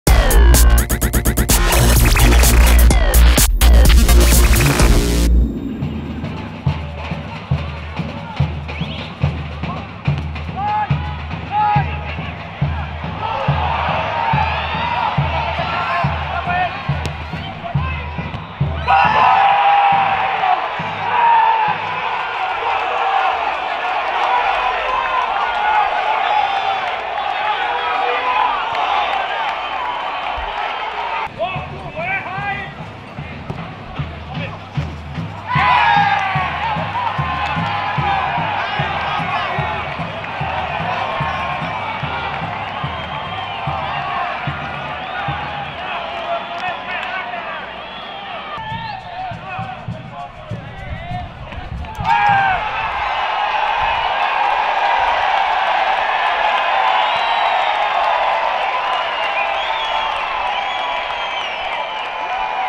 Os gols da partida - Novorizontino 0 x 3 Red Bull Bragantino
Pelo ângulo exclusivo da nossa câmera no estádio Jorge Ismael de Biasi, em Novo Horizonte, os três gols do Red Bull Bragantino neste domingo. Vamos, MassaBruta!